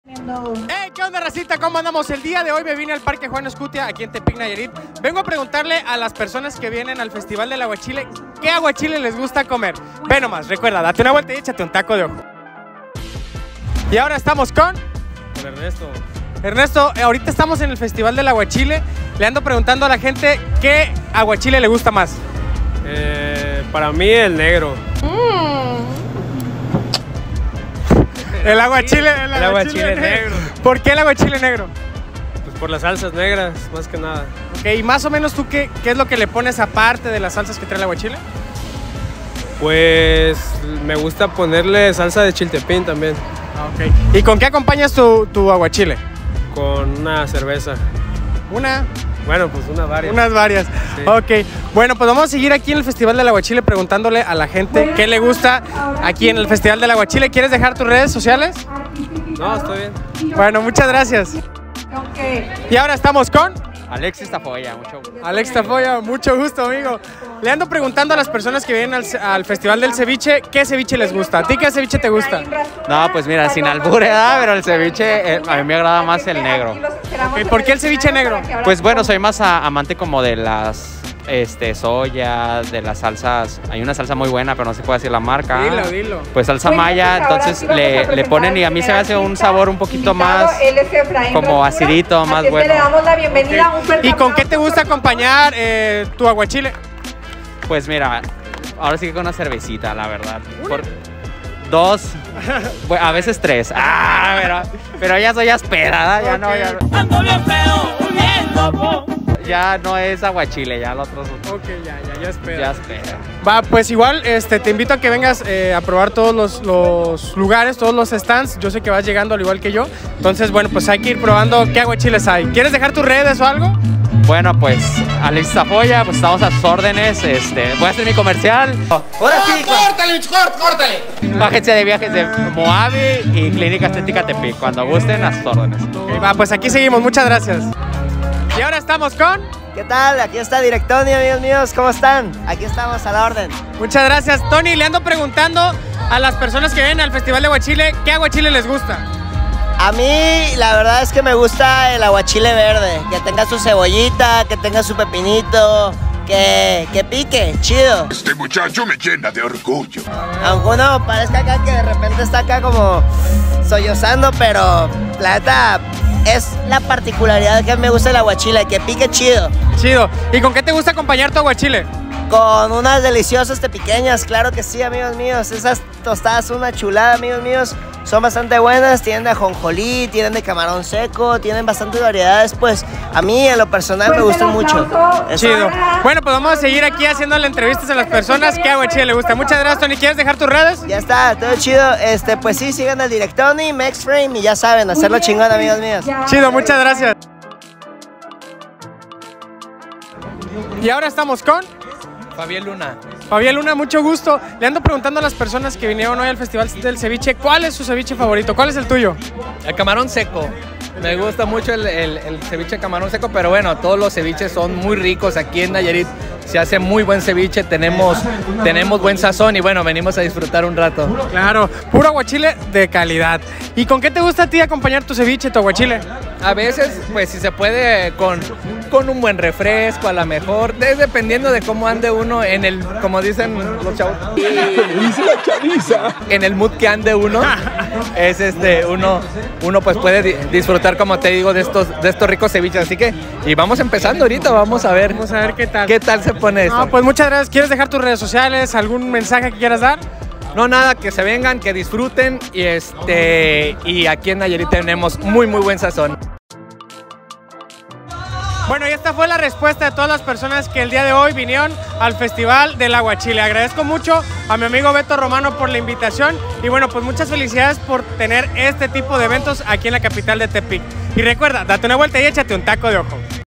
¿Cual es tu aguachile favorito?
Este 2 de abril acudí al primer festival del aguachile en Tepic. Esta fue la respuesta de algunos visitantes y me platicaron con que acompañan sus aguachiles